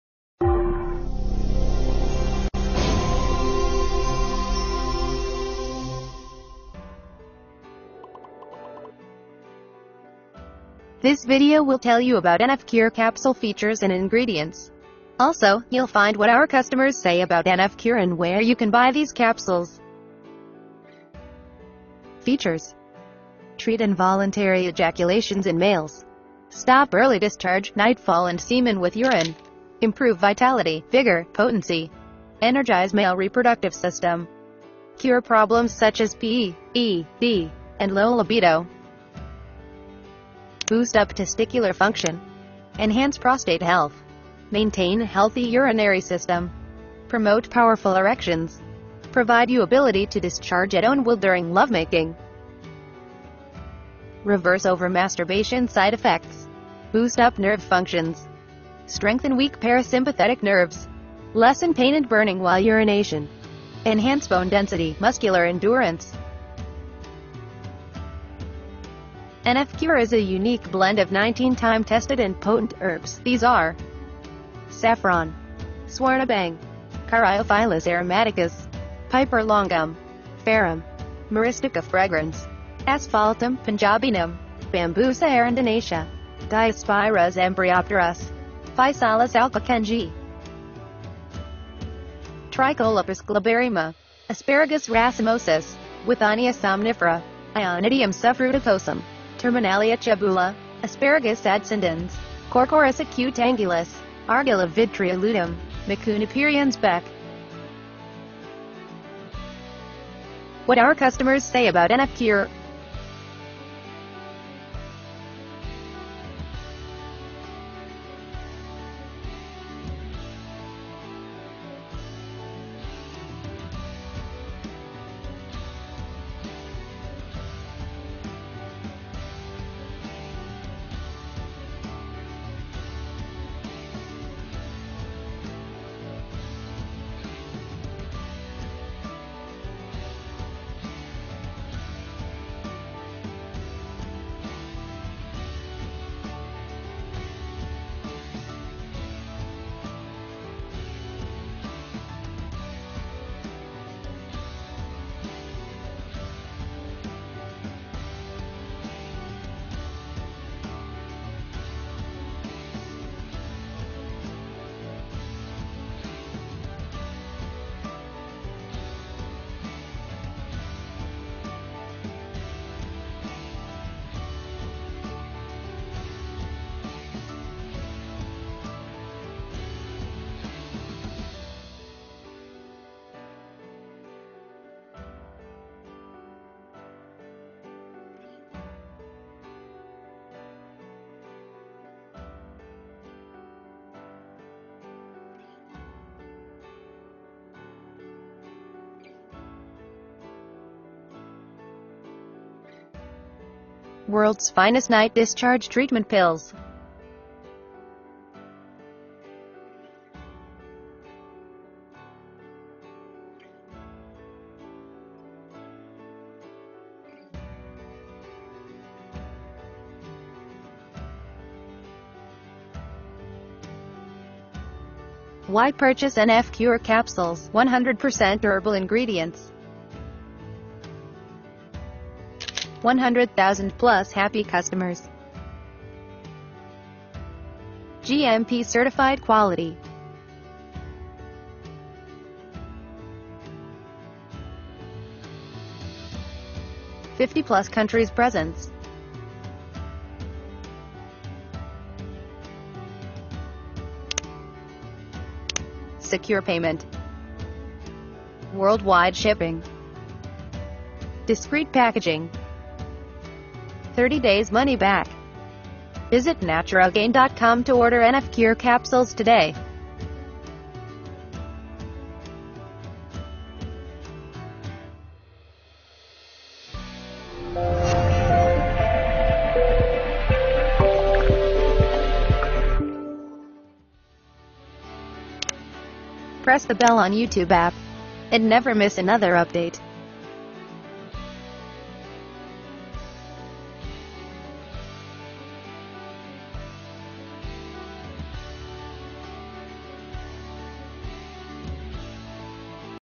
17 NF Cure Capsules Customer Reviews - What Real Users Are Saying? 🤷‍♂️
NF Cure Capsules Customer Reviews and Results 👍

This video will tell you about NF Cure capsule features and ingredients. Also, you'll find what our customers say about NF Cure and where you can get these capsules.

Must Watch the video before you buy NF Cure online. These real customer reviews of NF Cure capsules will help other users and buyers to make the right decision of choosing the best remedy to cure wet dreams problems.

NF Cure capsule is one of the most renowned and trusted herbal supplements for men's health problems such as nightfall, ejaculation during sleep, semen leakage, and masturbation side effects.

Men having any of the above-mentioned problems search for NF Cure capsules testimonials, reviews, and results of real customers. This video will end their search and help them in getting these pills securely and discreetly.